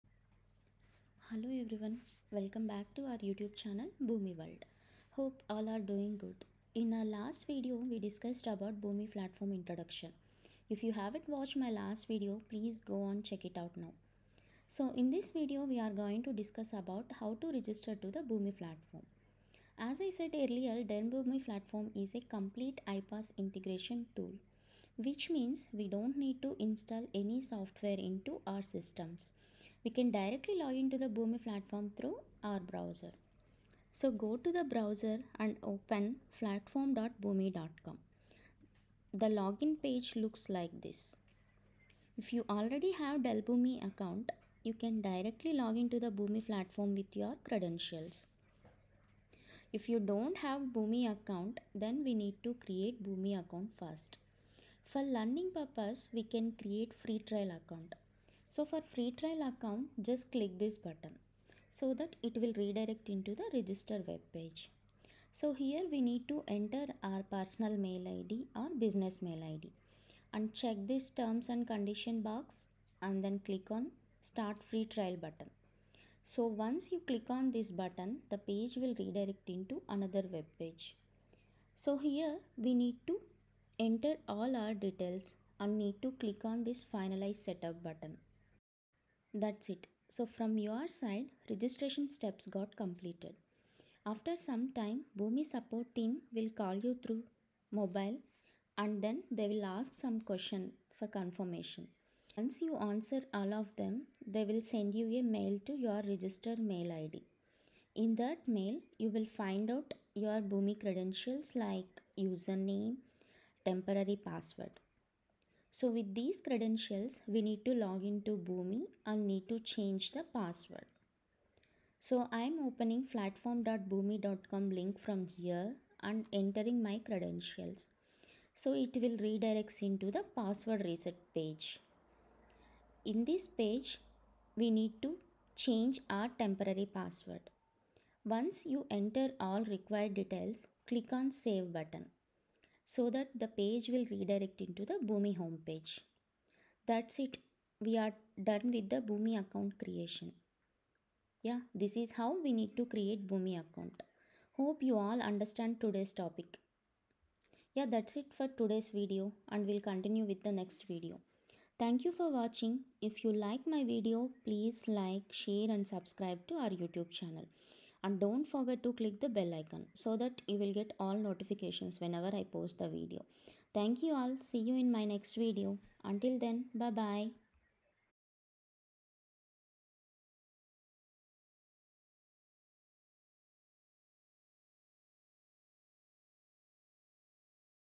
Dell Boomi Tutorial Day-2 | How to Create Boomi Account | Boomi World
Hello Everyone,
In this video we are going discuss about how to create Dell Boomi account from scratch.